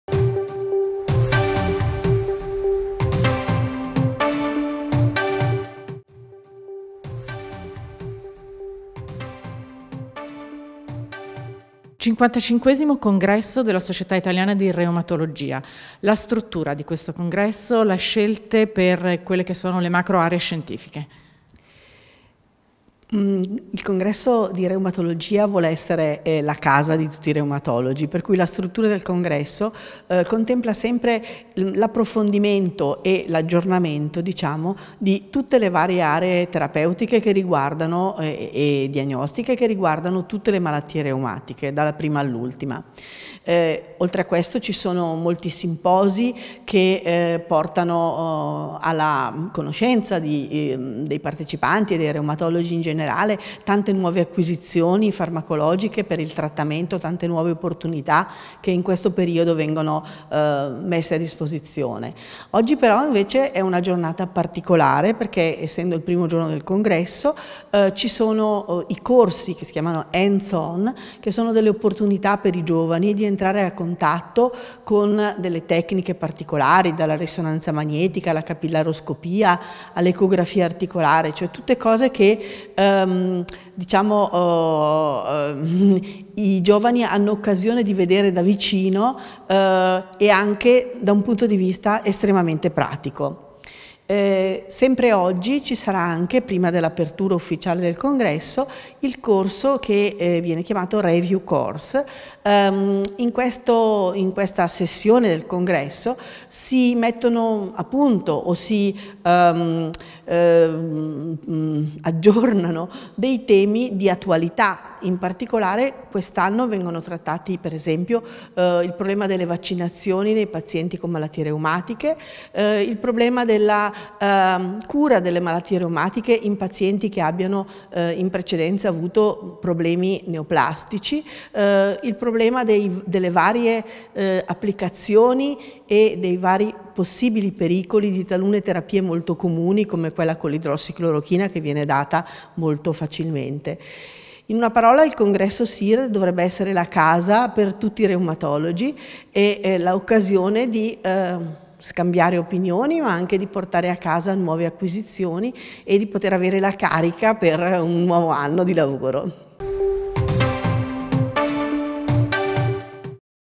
55° Congresso SIR - La Struttura del Congresso SIR 2018
Intervista alla Prof. ssa Angela Tincani nel corso del 55° Congresso Nazionale della Società Italiana di Reumatologia